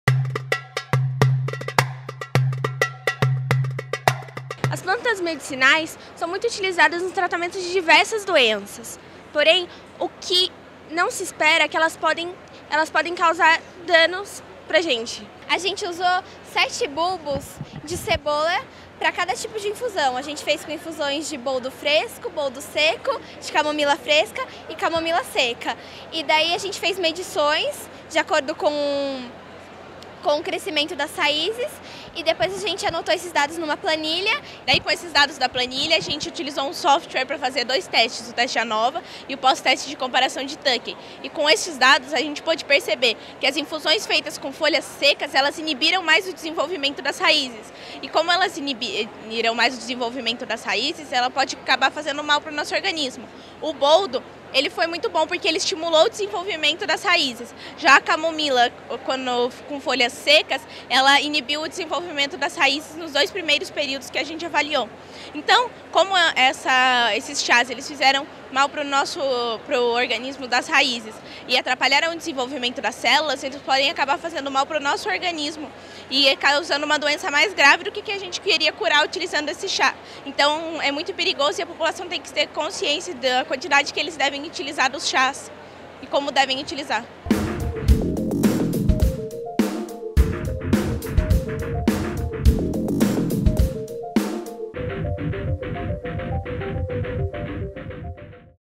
FEBRACE 2014 BIO 220
AVALIAÇÃO DO CRESCIMENTO RADICULAR DE ALLIUM
CEPA EXPOSTA A INFUSÕES DE PLANTAS MEDICINAIS

Flavia Maria Leme Munhoz
Vitória Avoni de Camargo
Caroline Pinheiro Santana
Ana Cristina Gobbo César (Orientadora)
Maria Gabriela Leme Munhoz (Coorientadora)

Instituto Federal de Educação, Ciência e Tecnologia do Estado de São Paulo
Bragança Paulista - SP